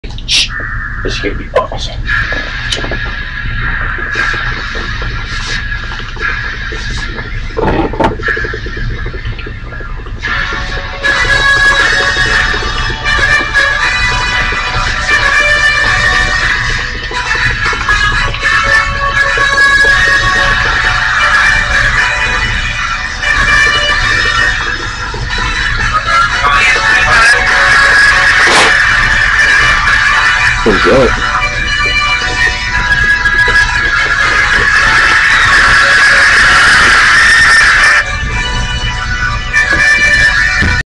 my shitty hookah
me and a few buds chillin, and kinda thought of makin a quick vid of me on my new hookah, with a very old, but epic song! peace guys